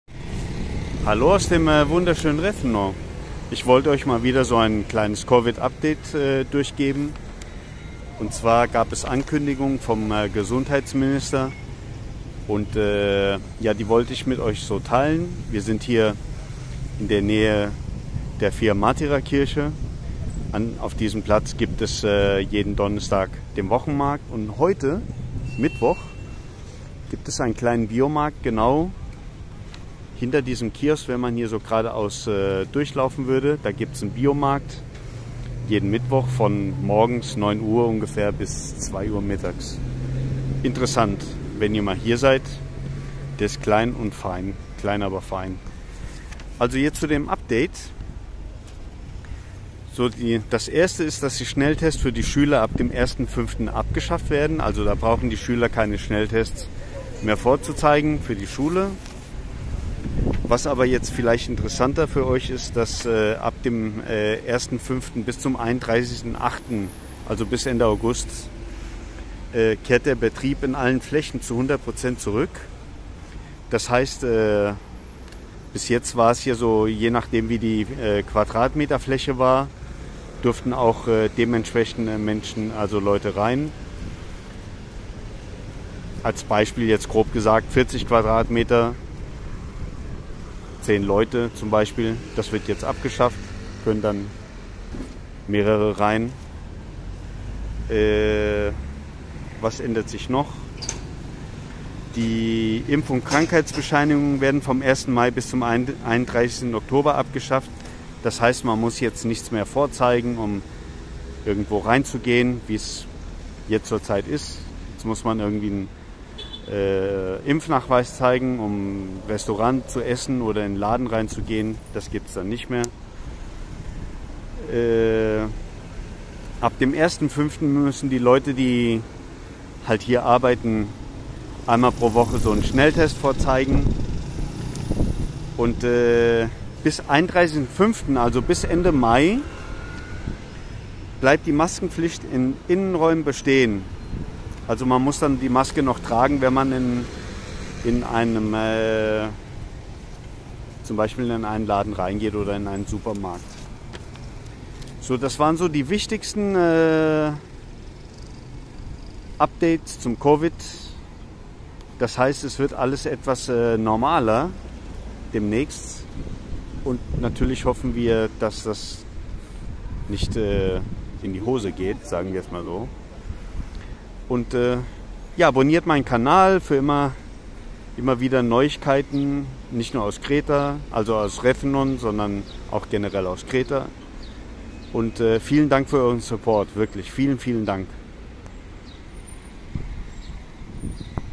KRETA - COVID UPDATE , was aendert sich demnaechst.. Crete 2022 Kreta 2022
Die Gesundheitsminister Plevris
Ankündigungen :

-Selbsttests für Schueler werden ab 01.05.   abgeschafft
- Vom 01.05. bis 31.08. gibt es keine einschraenkungen mehr fuer die Personenzahl zb in einem Laden oder Supermarket usw.
-Impf- und Krankheitsbescheinigungen werden vom 1. Mai bis 31. Oktober nicht mehr bennoetigt zum vorzeigen
-Ab dem 01.05.22 wird wöchentlich ein Schnelltest für den Zugang von
Ungeimpften zum Arbeitsplatz verlangt .
-Bis zum 31.05.22 bleibt die Maskenpflicht in Innenräumen bestehen.

Minister of Health Plevris –
-Self tests for students are abolished from 01/05
- From 01/05 to 31/08 the operation of all spaces returns to 100%
-vaccination and disease certificates are abolished from 1 May to 31 October.
-From 01/05/22 a rapid test is set per week for the access of the unvaccinated to the workplace.
-Until 31.05.22 the mandatory use of a mask indoors remains.

🌹Bitte Aboniert meinen Kanal fuer noch mehr aus Rethymno und Kreta!